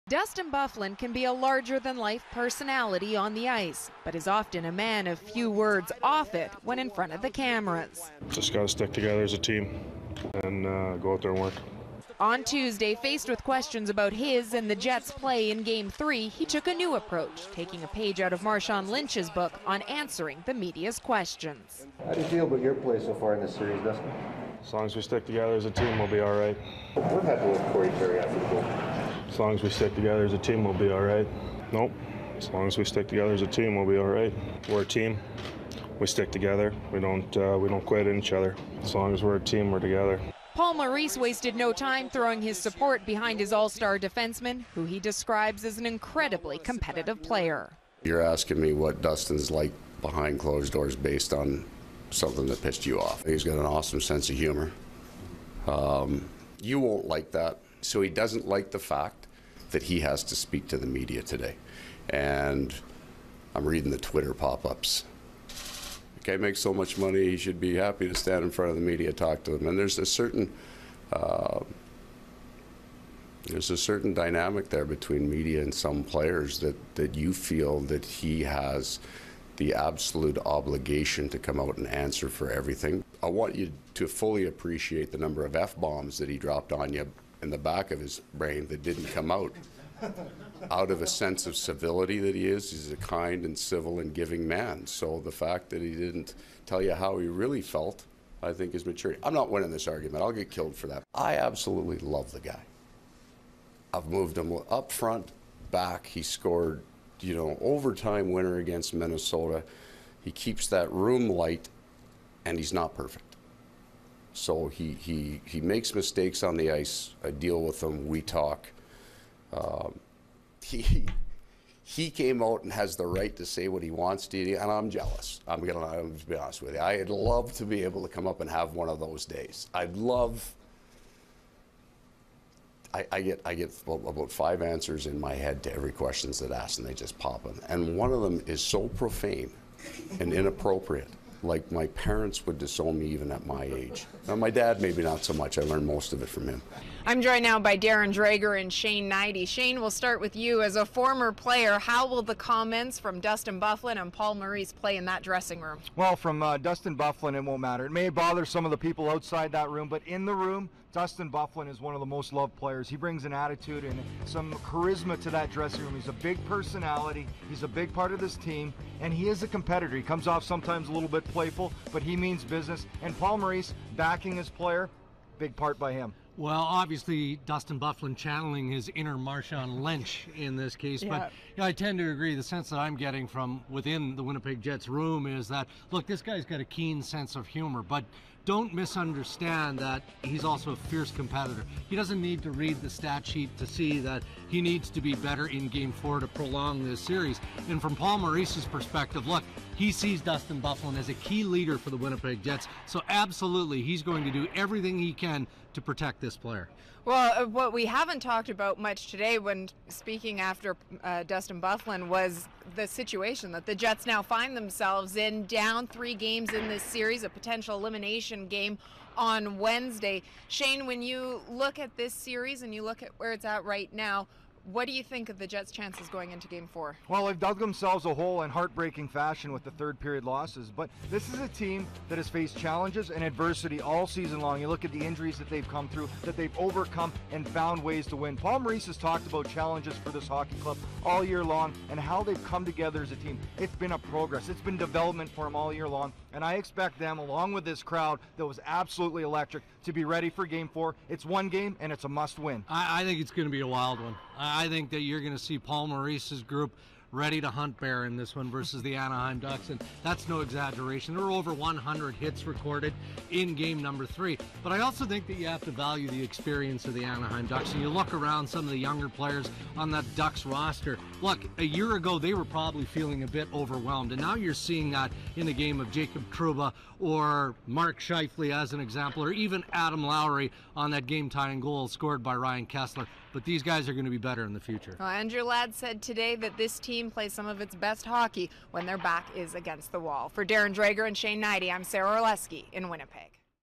Dustin Byfuglien has little to say with the Jets on the brink
After a disappointing loss that has the Jets on the brink of elimination, Dustin Byfuglien channeled his inner Marshawn Lynch, Sara Orlesky has more.